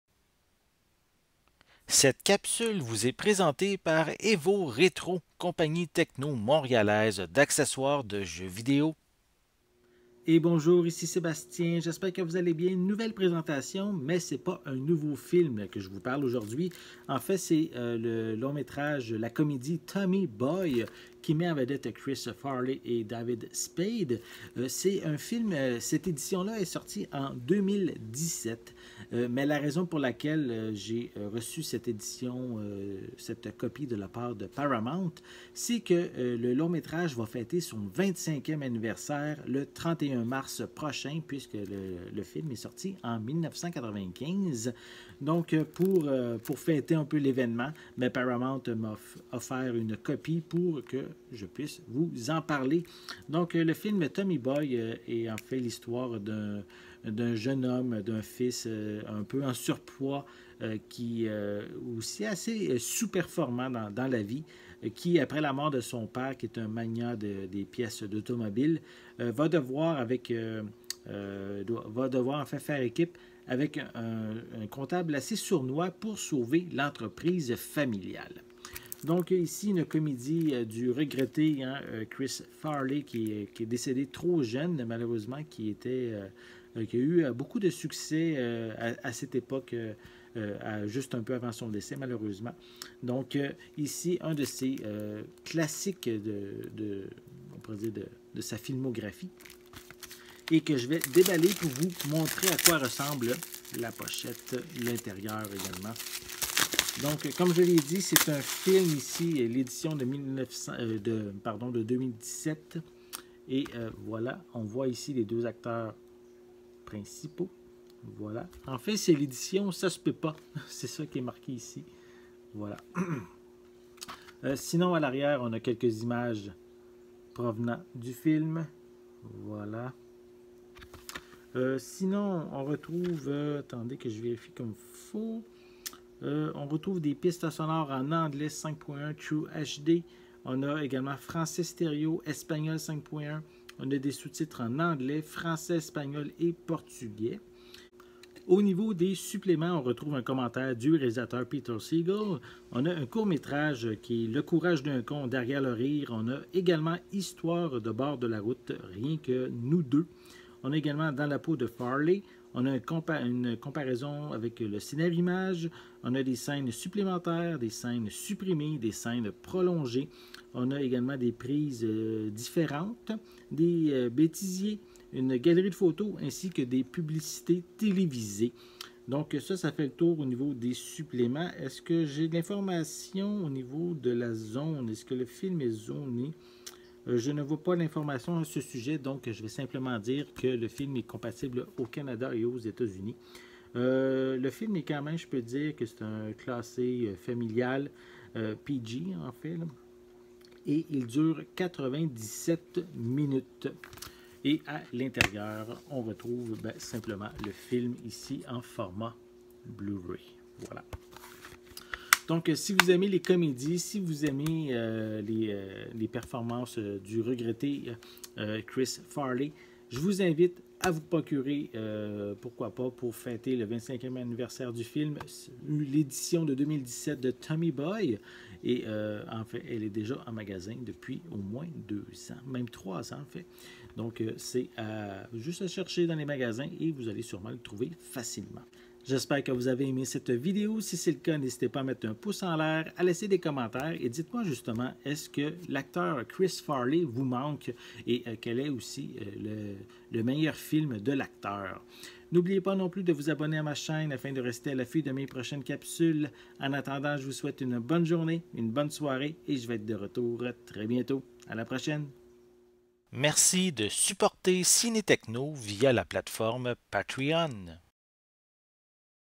Présentation (unboxing) du film Tommy Boy en format Blu-ray
Merci à mon partenaire Evoretro

Réalisé par Peter Segal et mettant en vedette Chris Farley, David Spade et Brian Dennehy. Distribué par Paramount et disponible en magasin depuis le 7 février 2017.

Tommy Callahan, fêtard invétéré et perpétuellement fauché, finit, au bout de sept années, par obtenir son diplôme... et un boulot peinard chez Callahan Pièces Auto. Lorsqu'il rentre chez lui, une autre bonne nouvelle attend Tommy : son père va épouser un véritable "canon" et Tommy aura enfin le demi-frère dont il a toujours rêvé.